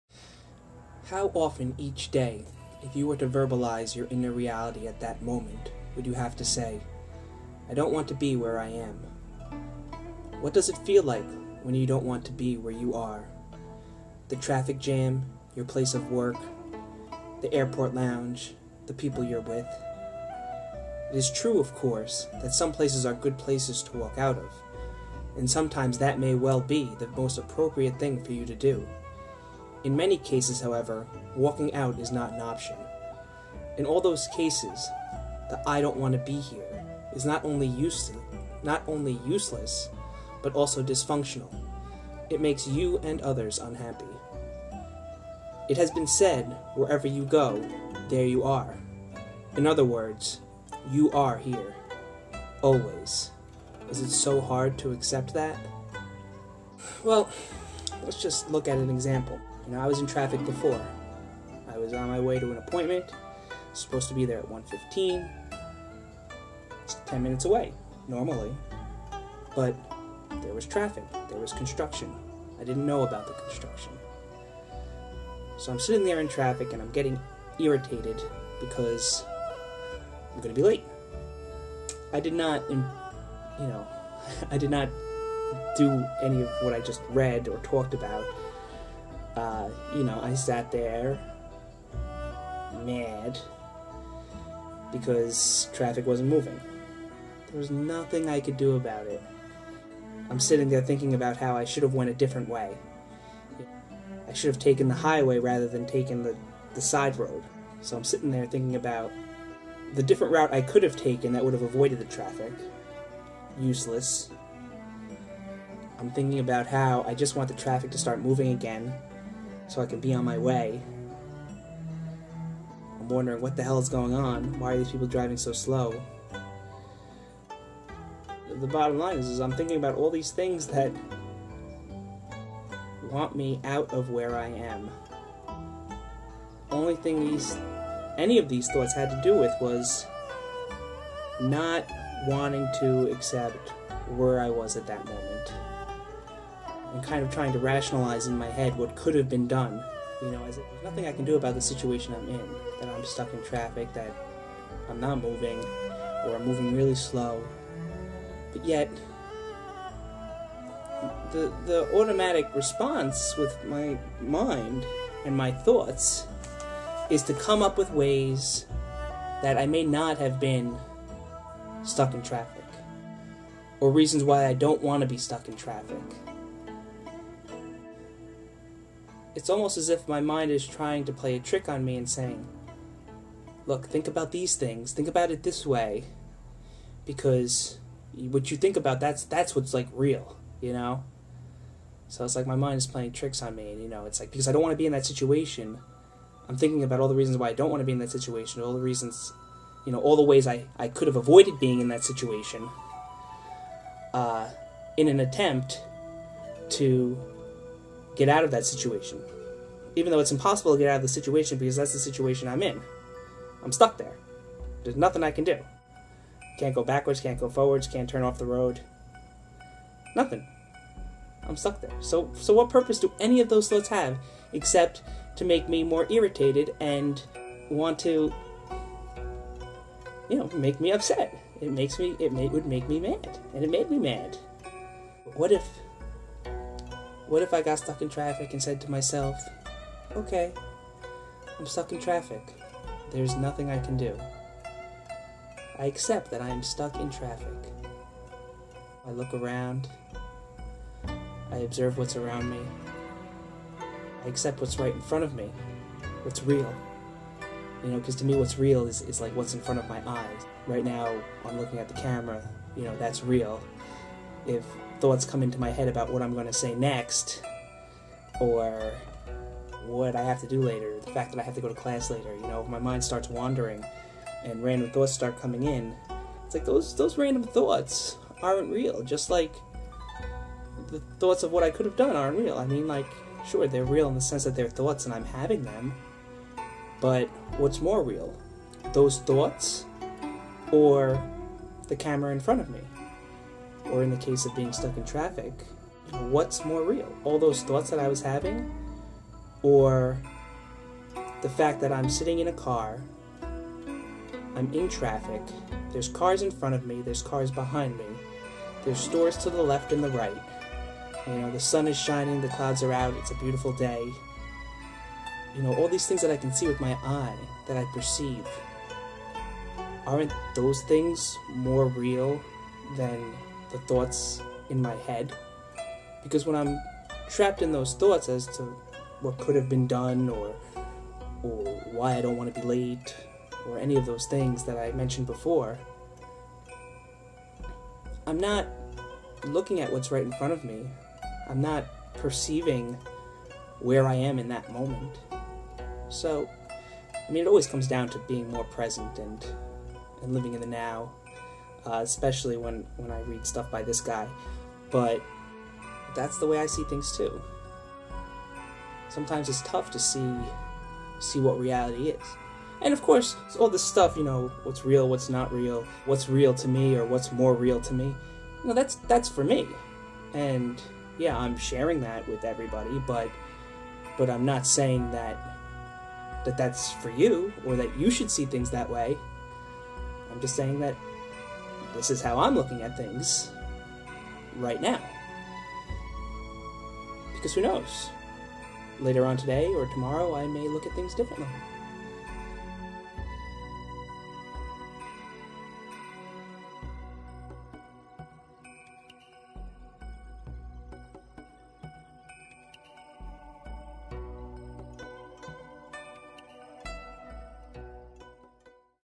Stillness Speaks 09-09-09
So this is me talking about a passage from "Stillness Speaks" by Eckhart Tolle, it has to do with being present where you are, and accepting situations that you are in where it is counter productive to be thinking about how you don't want to be in the situation.  Im just sort of rambling but maybe it will make sense to somebody out there.